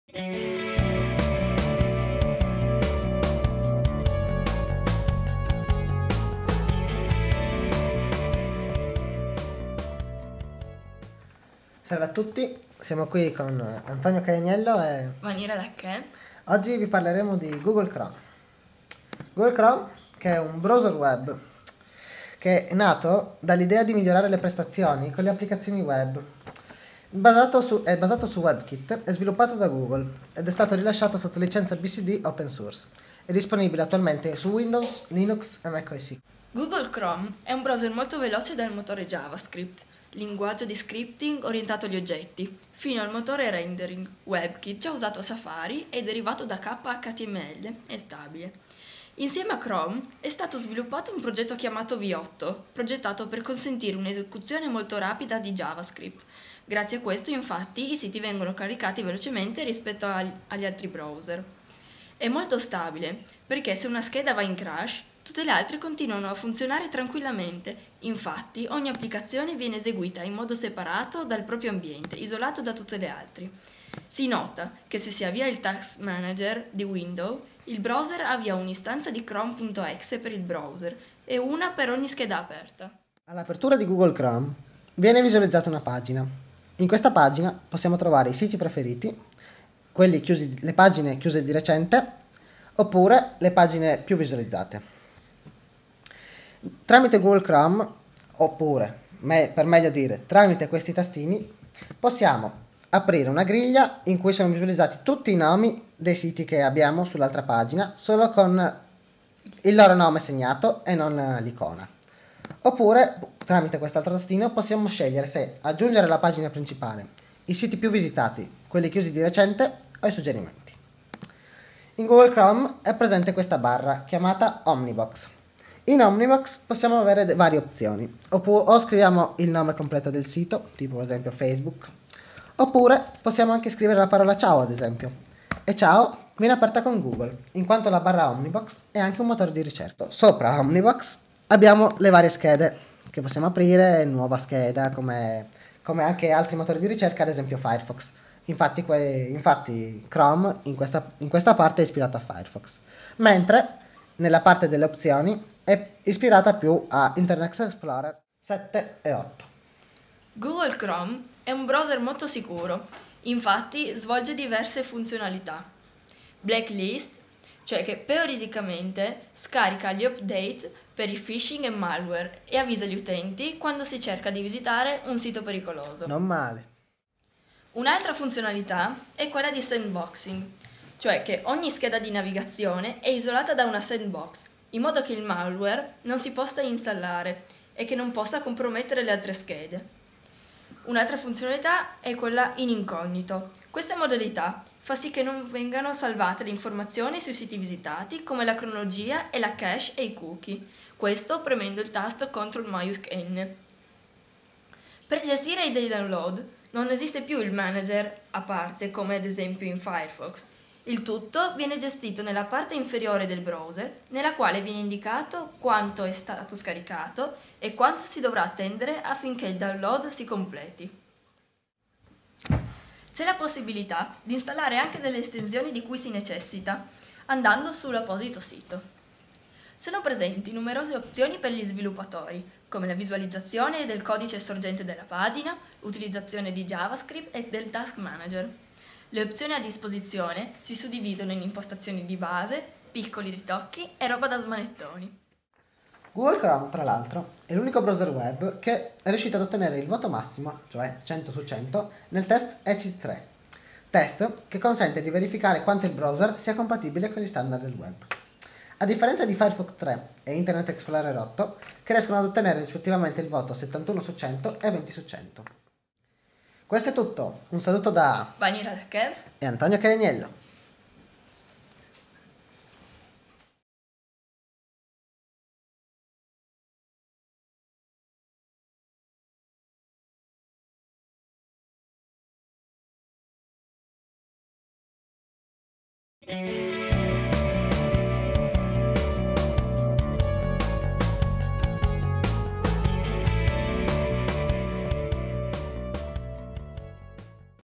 Videolezioni sul software: Google Chrome (BY ISITGOONAIR)
Antonio, Vanira ed Alberto della classe 4A informatica abacus dell'ISIT "Galilei-Fermi-Pacassi" di Gorizia illustrano ai microfoni di Isitgoonair, le caratteristiche e l'utilizzo di Chrome, il noto browser di Google